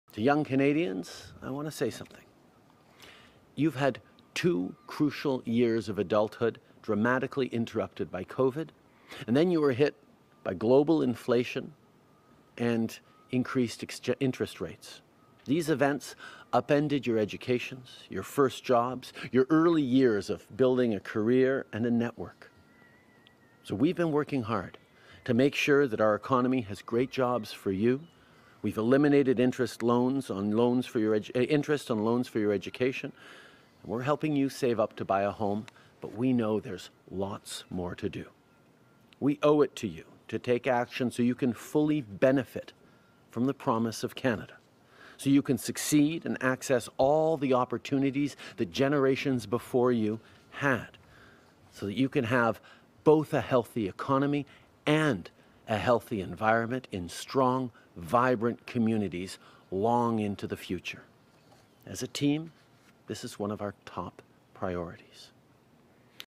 CANADA HOUSING CRISIS | Watch Justin Trudeau's message to millennials
Prime Minister Justin Trudeau spoke directly to younger Canadians who have been dealing with years of social and economic turmoil.